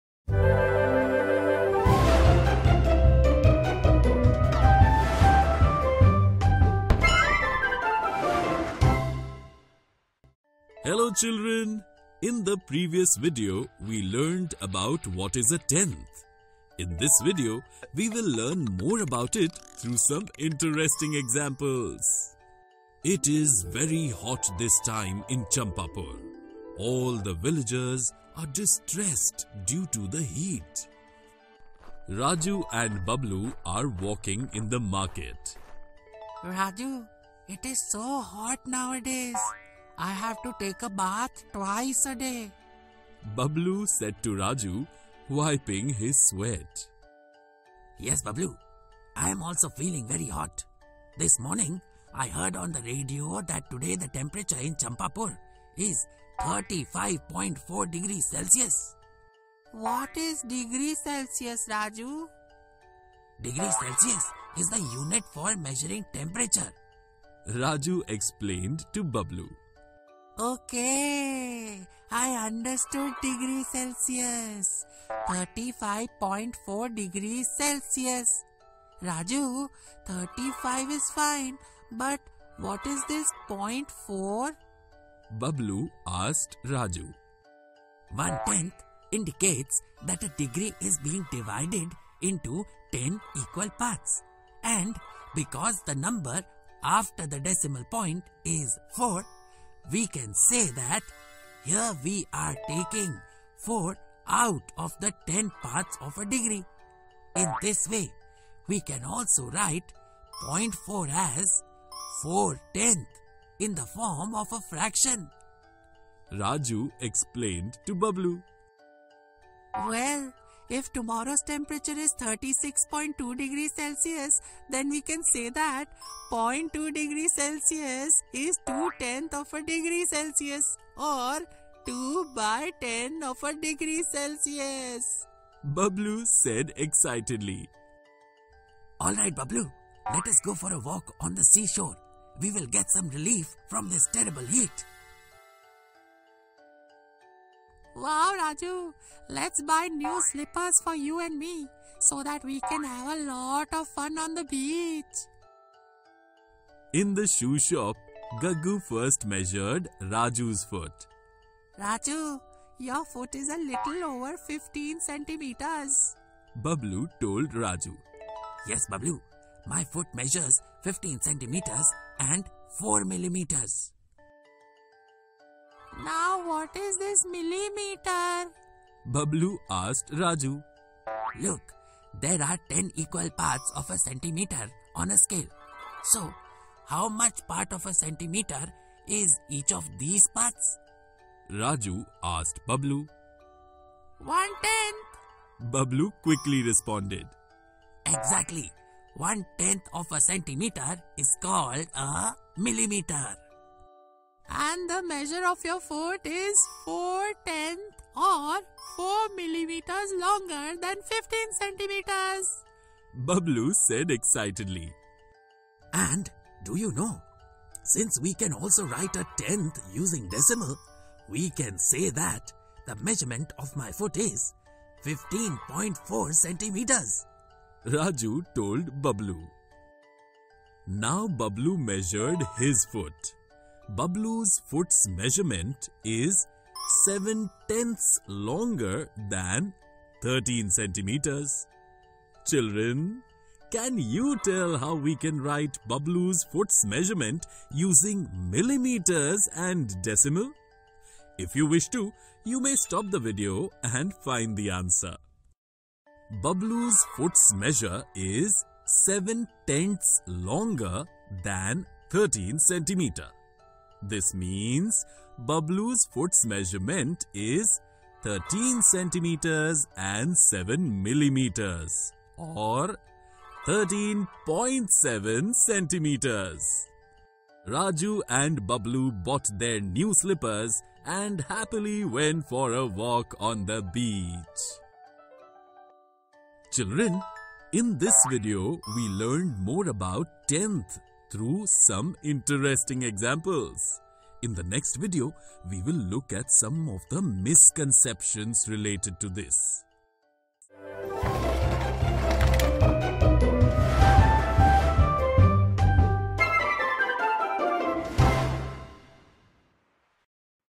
Decimals: A tenth | Part 2/3 | English | Class 5
Decimals: A tenth  | Part 2/3 | English | Class 5 | NCERT

Decimals: A tenth  | Decimals | Grade 5 | NCERT | Tenth and hundreds

Welcome to part 2 of Decimals: A tenth . In this video, we will discuss various examples of Decimals: A tenth .